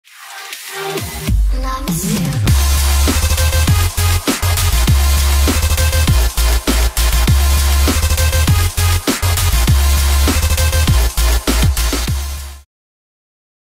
FREE Intro Template [Smoke Magic] from Make3Design for TheAd4m (font Spider Monkey)
Toto je intro je vyrenderované z šablony a není nijak upravené.

Původní autor této šablony je "Make3Design" a ten komponenty do intra také od někoho převzal a upravil... To je holt koloběh života. Takže si prosím odpusťte komentáře, že nejsem autor (ano, nejsem), že nejsem originální (ano, nejsem) a že kopíruju (ne, nekopíruju, využívám pouze to, co je k dispozici).